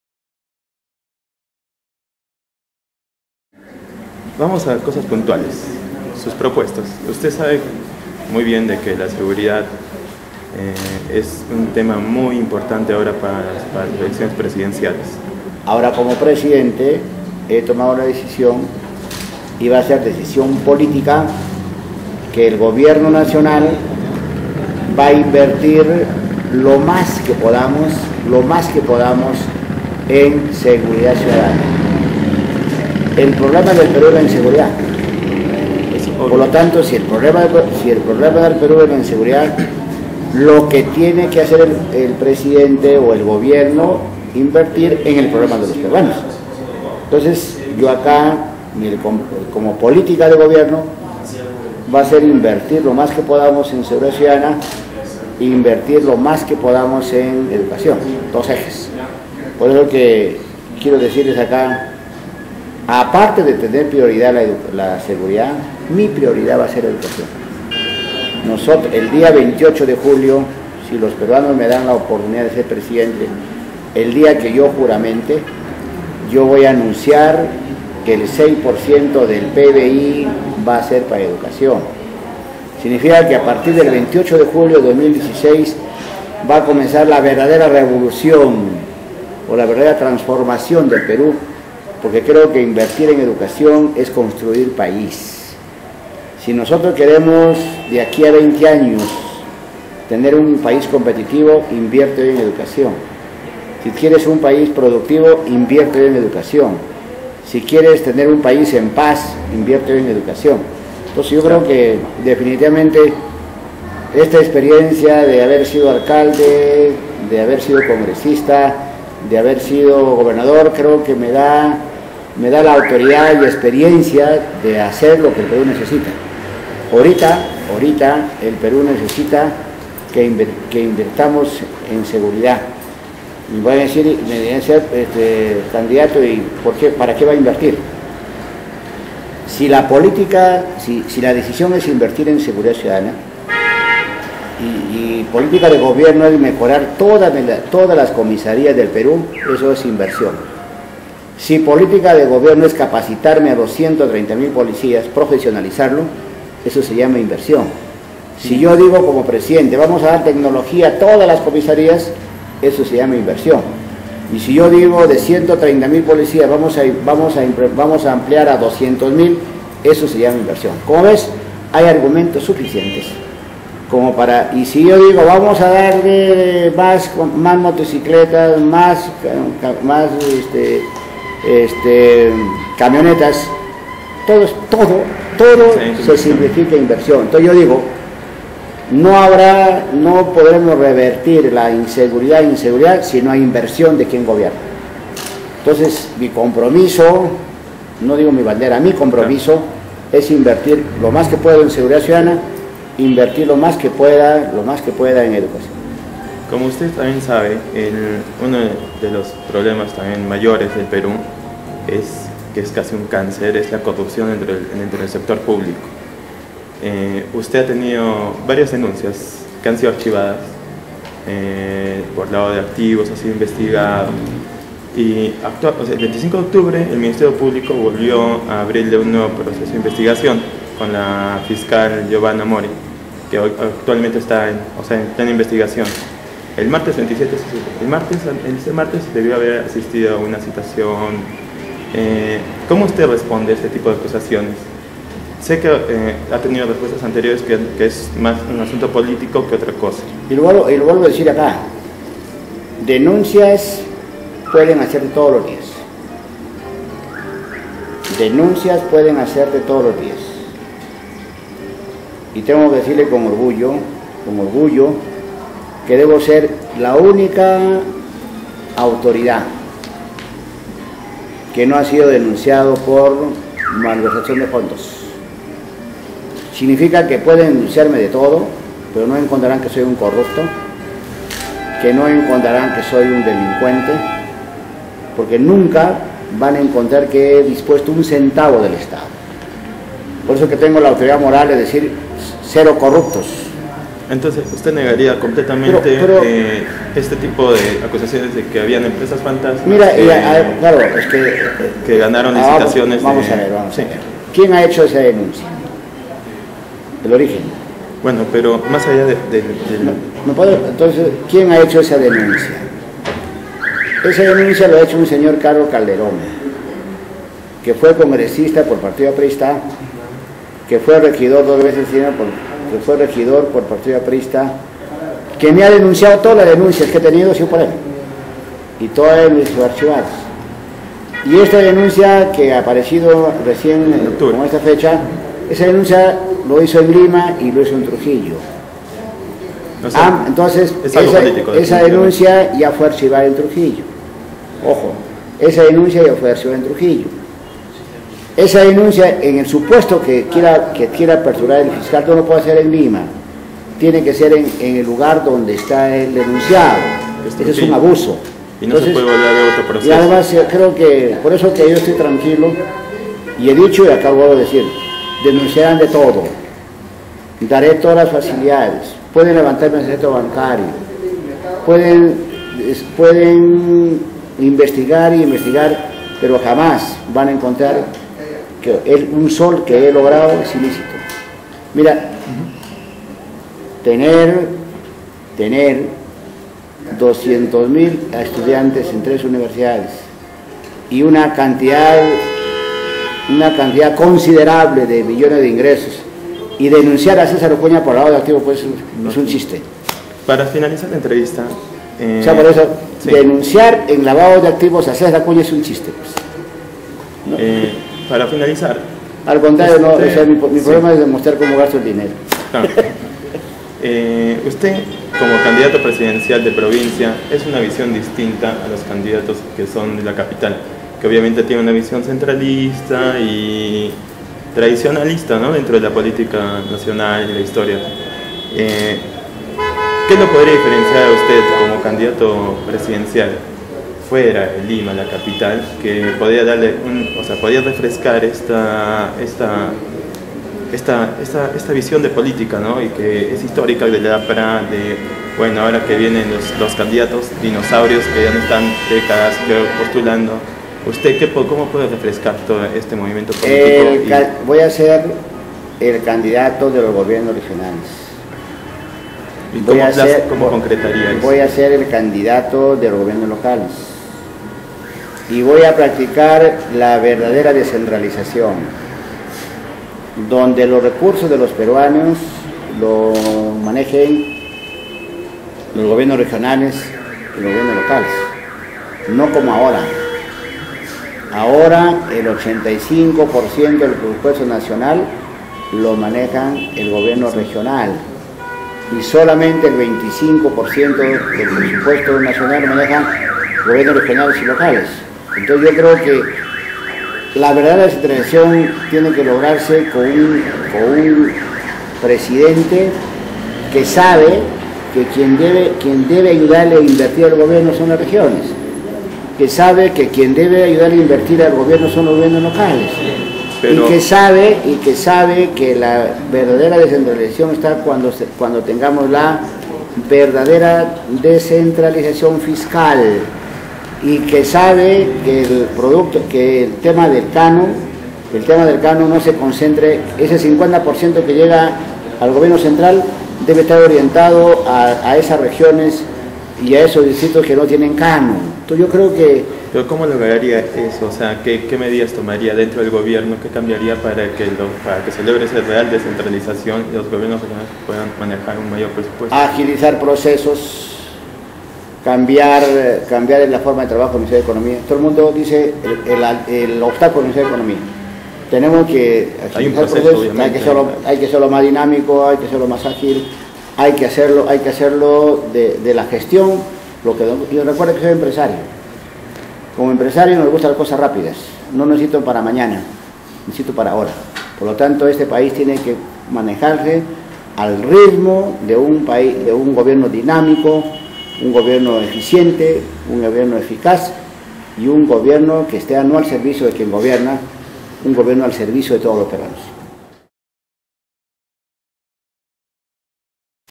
César Acuña, candidato presidencial de APP, habla de Seguridad y corrupción
El candidato de Alianza para el Progreso, durante su última visita a nuestra ciudad, converso brevemento con El Búho sobre estos temas.